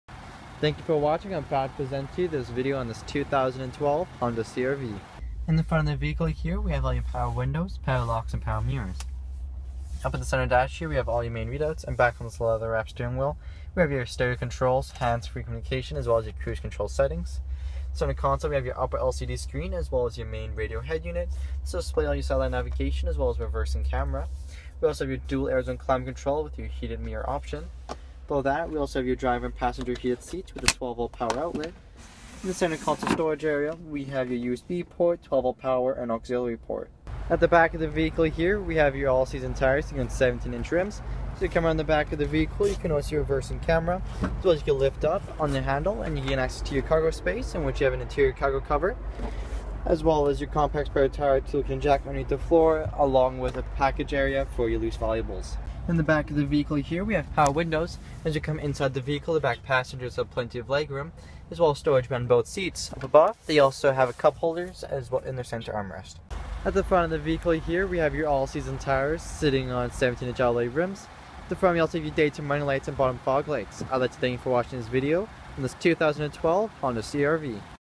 2012 Honda CR-V AWD 5dr Touring | Alberta Honda
This is a 2012 Honda CR-V AWD 5dr Touring with A/T transmission Black[NH731P,Crystal Black Pearl] color and Beige interior color.  This video is recorded and uploaded by cDemo Mobile Inspector